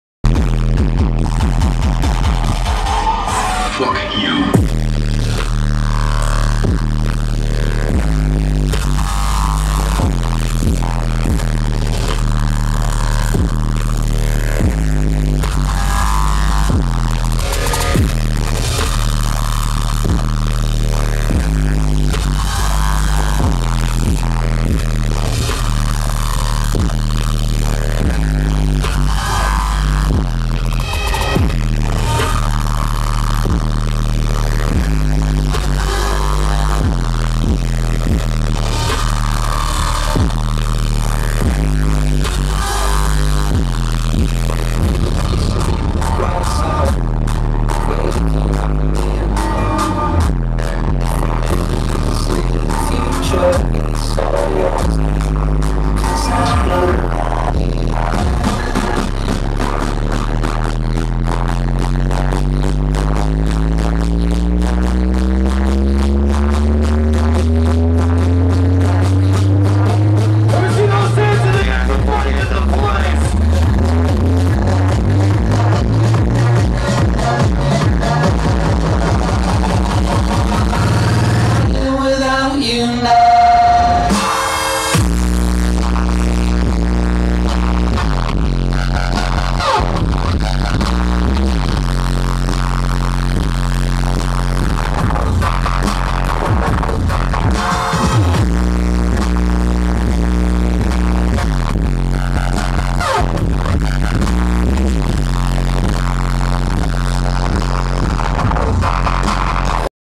Excision @ Nocturnal 2014
Bassrush Stage with Excision @ Nocturnal Wonderland 2014 CA 1st Night.